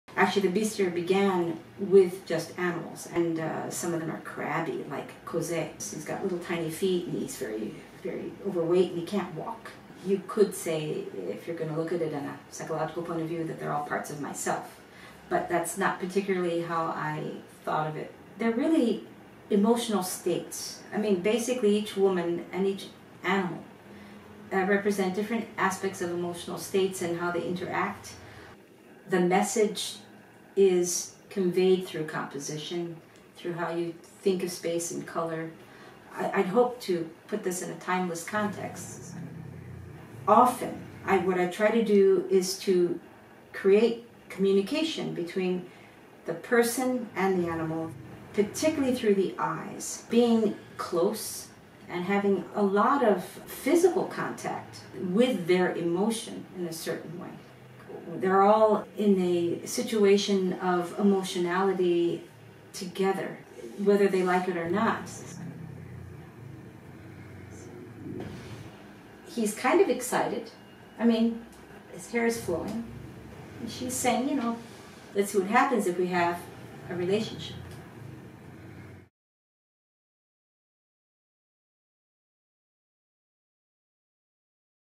"Bestiary" painted by Rebecca Hayward
In this video the Florence based painter Rebecca Hayward talks about her Bestiary series in which she explores the relationship between a woman and an animal side of her portrayed in a variety of ways. The series have been created over the course of several decades.

The video is directed, shot and edited by Karin Grand as a part of a mini serie on Hayward's work.